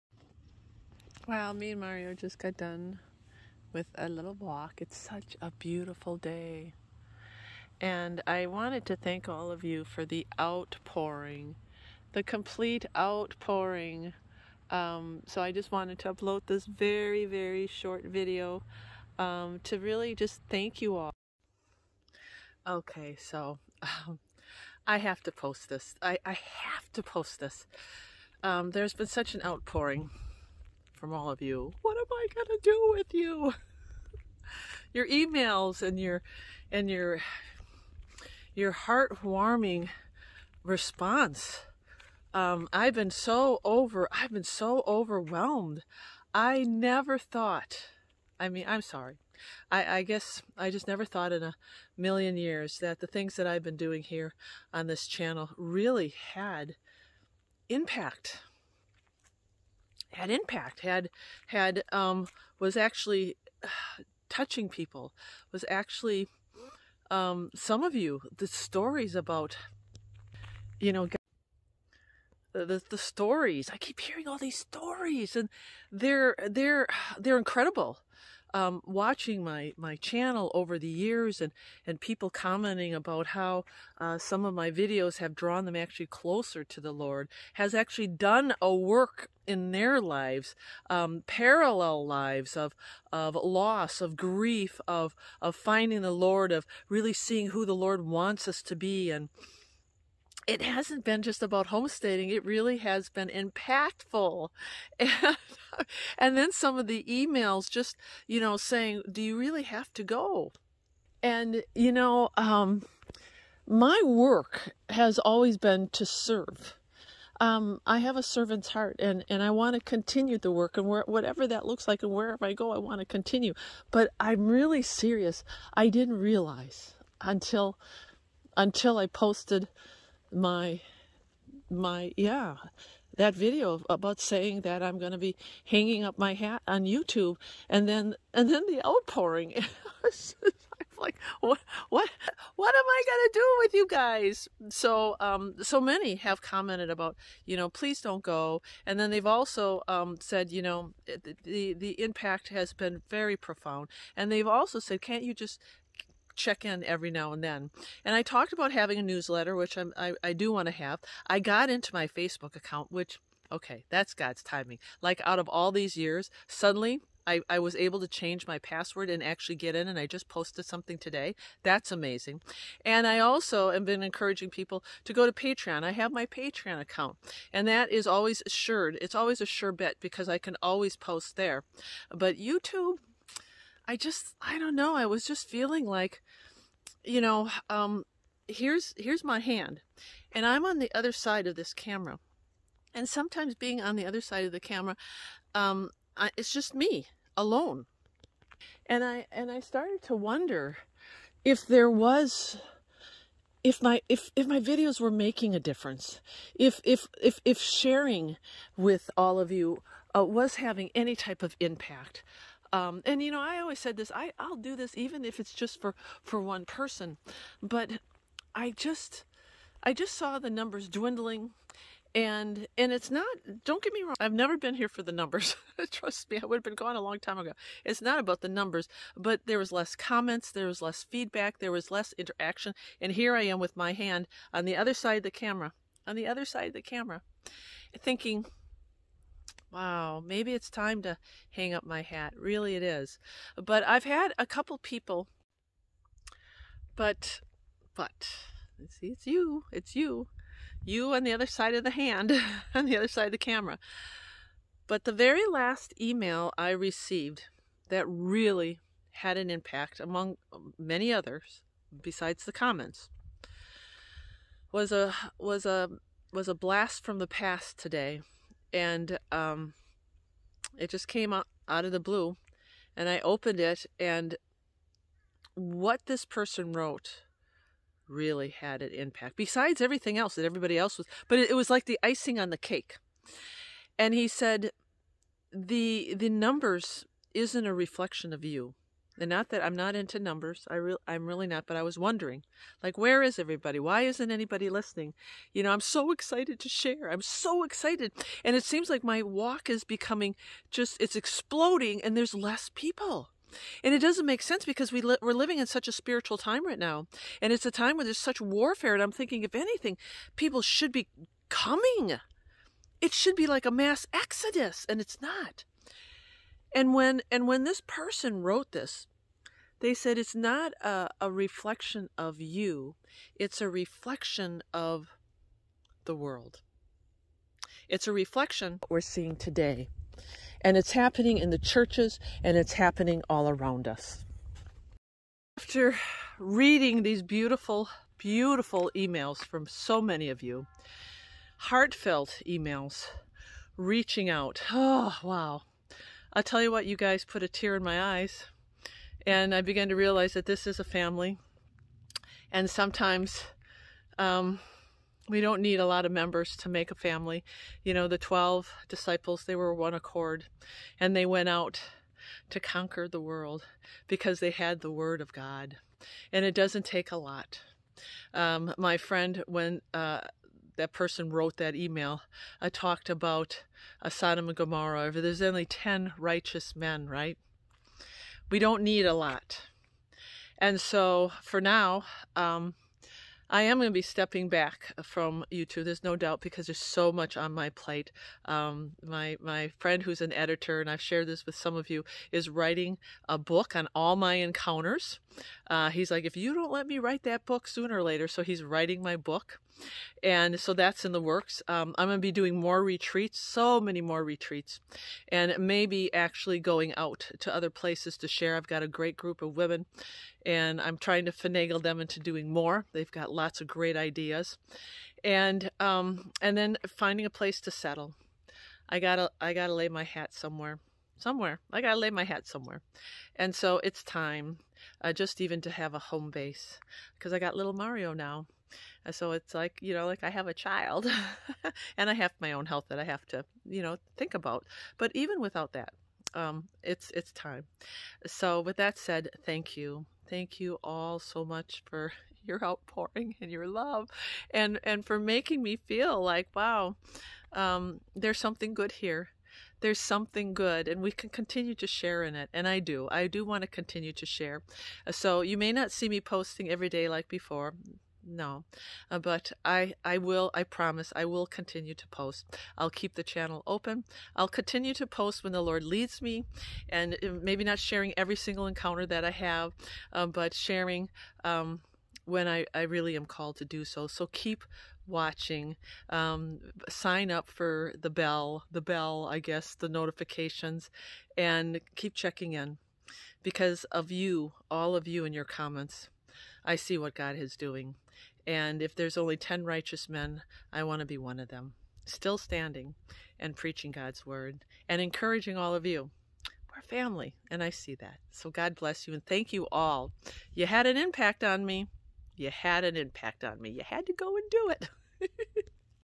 WHAT HAS YOU TUBE DONE TO STARRY
🤭 what have you guys done? I cannot believe the outpouring from my last video about me stepping down. so many people responded. so many people wanted me to stay so many people sharing how my channel has made a difference. I know this has been quite the journey for me and so many people have become just like family. I feel connected. but I guess I never thought I never thought there was an impact until the other day when I posted that video.

truly it's not that I was feeling that I needed to step down because things are so busy. but I just felt like I'm not sure maybe it wasn't worth it anymore. but truly the outpouring really showed me something different. I really appreciate each and everyone of you and I appreciate you all coming out to tell me how you felt. but that said I won't hang up my hat quite yet. but I won't be posting as much.
😉I encourage people to watch my Facebook page and to sign up for patreon every little bit helps. and I appreciate all the emails for the newsletter I'll definitely keep all those and the people offering me a place to stay if I may ever come their way wow this with tears my eyes I just can't believe it.
🥲so thank you for the outpouring I am forever grateful really like forever grateful